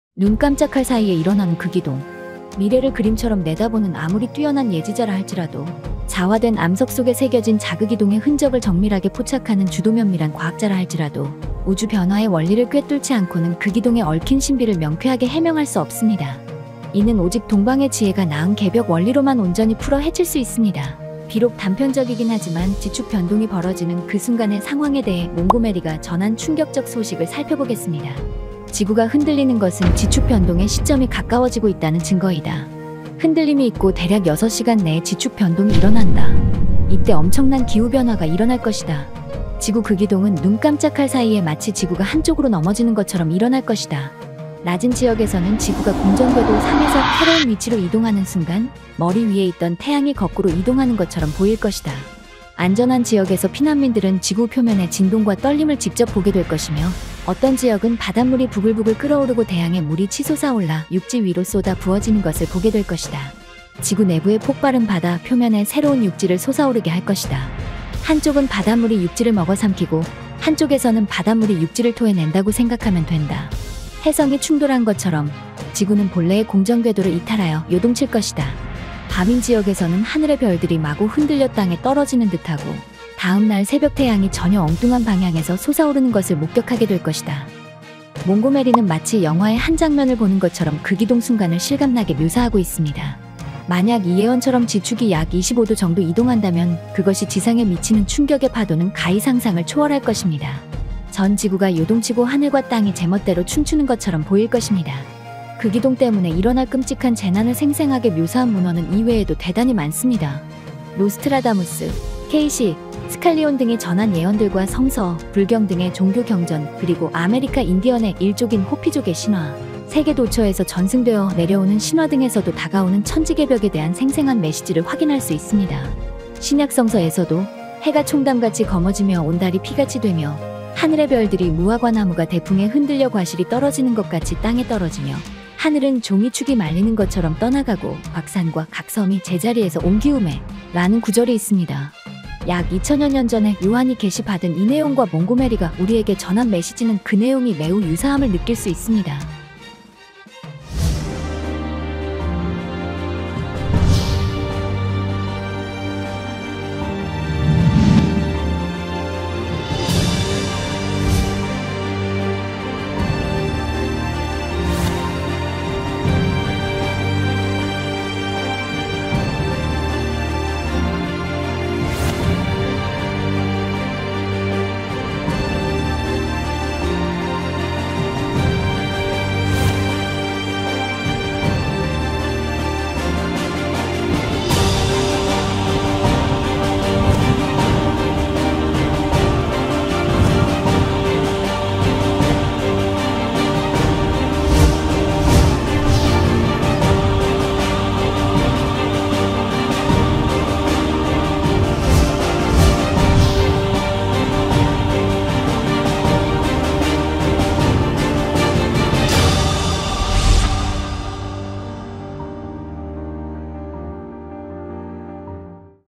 [1분개벽] 눈깜짝할 사이에 일어나는 지구궤도이동 루스 몽고메리 예언 이것이 개벽이다
영상이 유익하셨다면 좋아요,구독 꾸욱~ 눌러주세요^^
더 궁금하시거나 소책자를 받아보고 싶은 분은